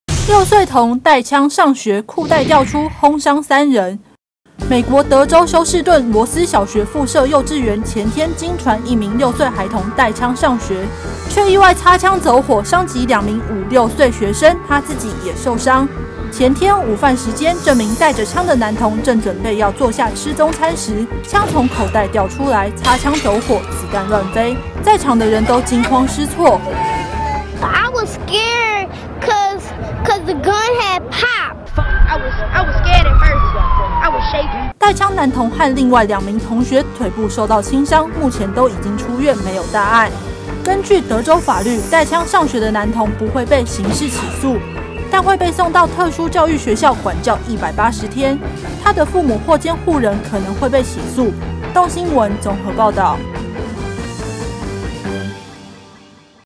Six-year-old injures three classmates with gun
A six-year-old in Houston brought a loaded gun to school. The gun went off in the cafeteria and injured the boy himself and two other kids in school. They suffered minor injuries on their legs and have left the hospital. The boy won't be prosecuted but his parents might face charges for this incident.